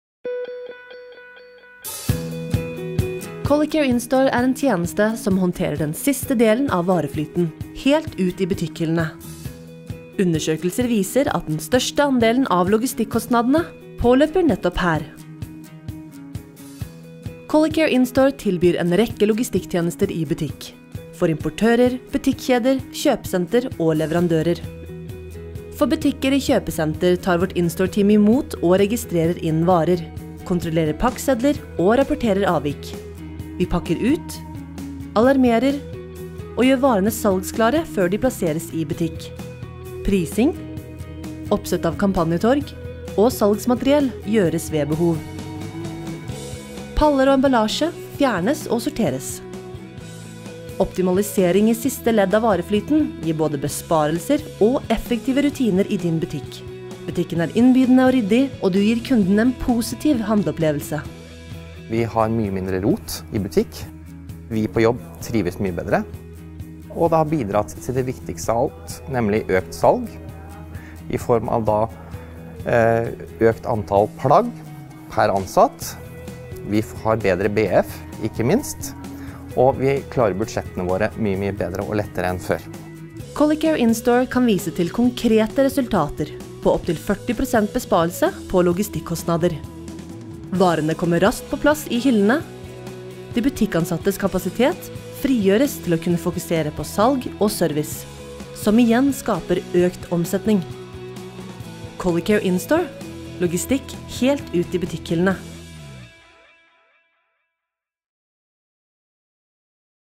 InStore / ColliCare Logistics
ColliCare InStore is a solution where we handle your entire supply chain - from manufacturer to placing your prdoucts on shelves in the shop. For your shop's employees the customer should be in focus, not unpacking goods when there are customers in the shop. Save by choosing a complete logistics solution.
ColliCares InStore er et konsept der vi tar oss av hele vareflyten din – fra produsent til ferdig utpakkede varer i din butikk. For dine butikkansatte er det kunden som skal være i fokus! Da er det ikke optimalt å være opptatt med utpakking av varer når kundene er i butikken. Spar på å velge en total logistikkløsning.